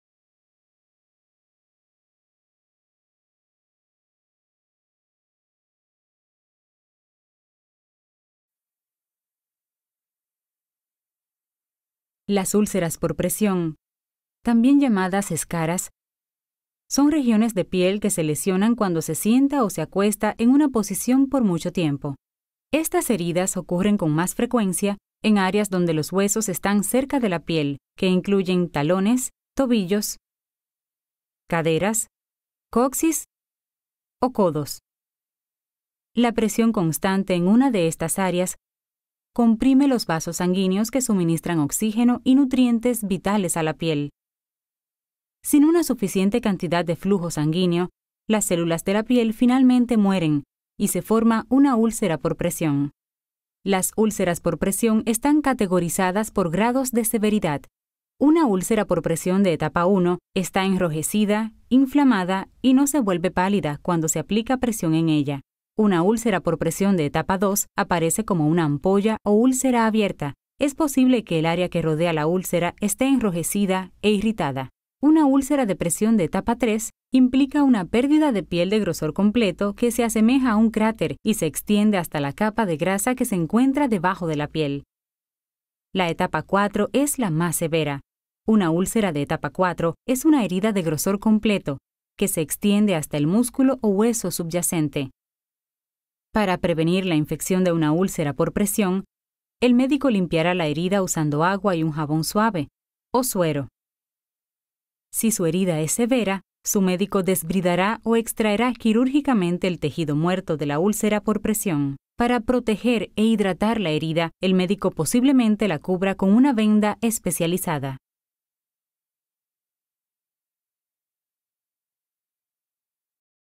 Úlceras por presión
Nucleus Medical Media creates medical animations in numerous languages for patient education and content marketing purposes.

Esta animación en 3D brinda una visión general de cómo y dónde se pueden desarrollar las úlceras por presión en el cuerpo. Las úlceras por presión, también llamadas escaras, son regiones de piel que se lesionan cuando se sienta o se acuesta en una posición por mucho tiempo. También se muestra las cuatro etapas diferentes de formación de úlceras por presión, junto con algunas opciones de tratamiento.